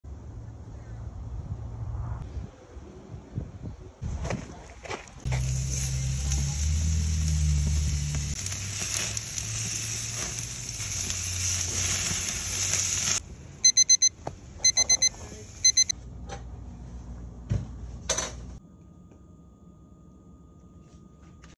Grilling in an Ice Storm
Love me some summertime BBQ, but Michigan keeps sending us these weak Ice Storms. Winter is getting in my way of BBQ. Today, we had an Ice Storm and I felt the need to throw some New York Strips on the grill. - Sean Whitcomb

Youtube Shorts